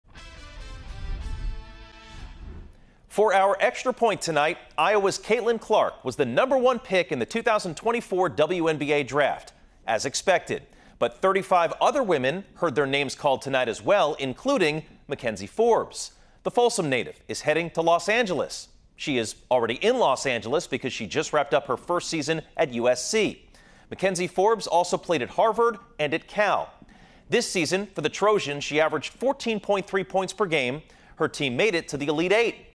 Folsom native McKenzie Forbes drafted in third round by Los Angeles Sparks
McKenzie Forbes played one season at USC after transferring from Harvard (and before that, Cal). Forbes is a former standout at Folsom High School where she was a McDonald's all-American.